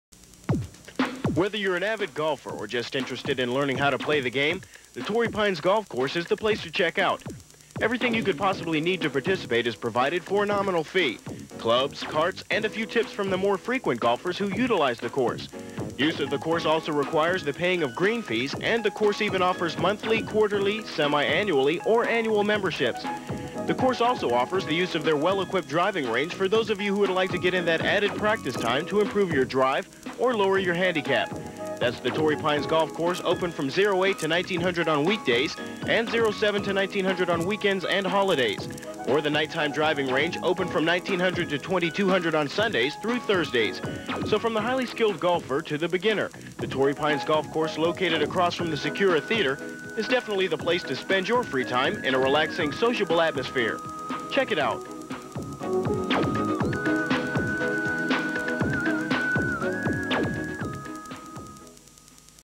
MCAS Iwakuni Japan Torii Pines Golf Spot
Spot Produced in support of MCAS Iwakuni, Japan's Torii Pines Golf Course. c. 1990